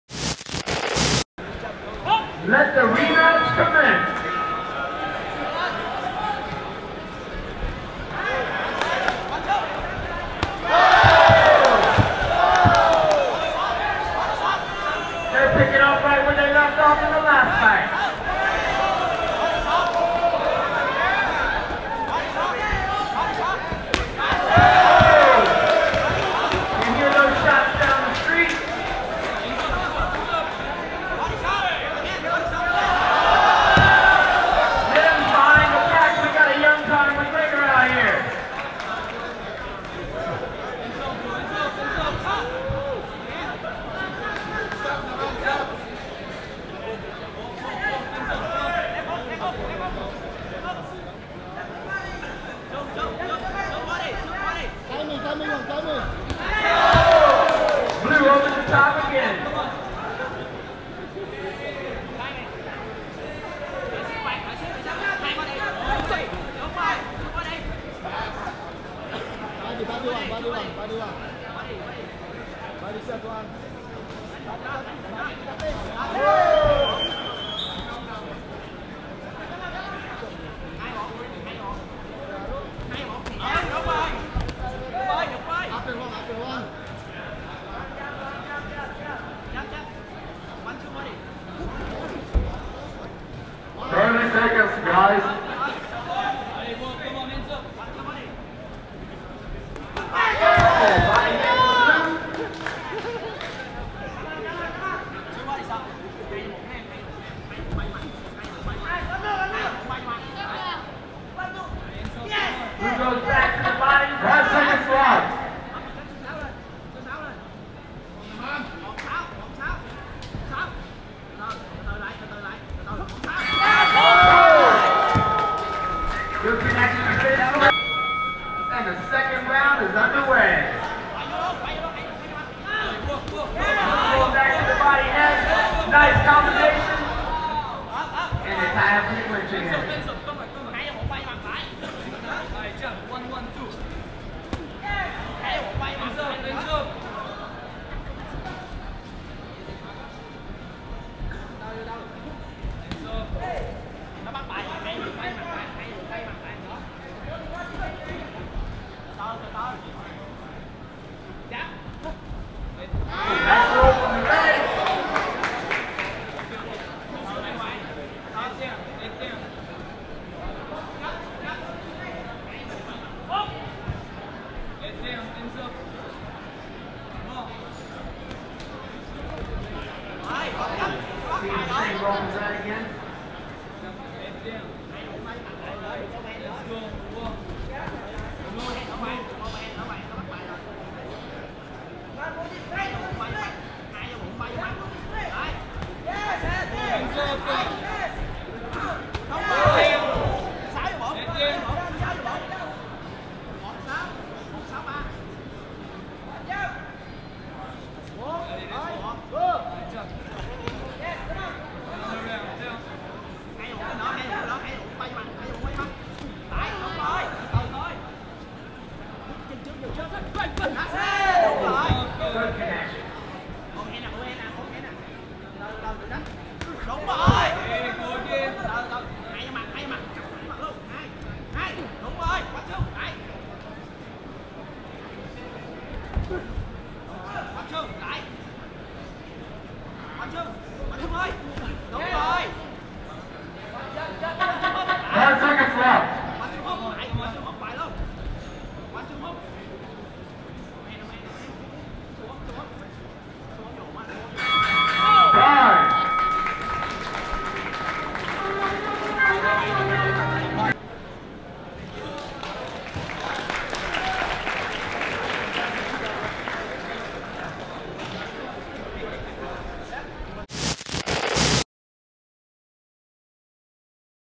Saigon Interclub Boxing 3 @ Saigon Sports Club | LE MINH HOANG vs LE NAM ANH
Saigon Interclub Boxing @ Saigon Sports Club Ho Chi Minh City, Vietnam.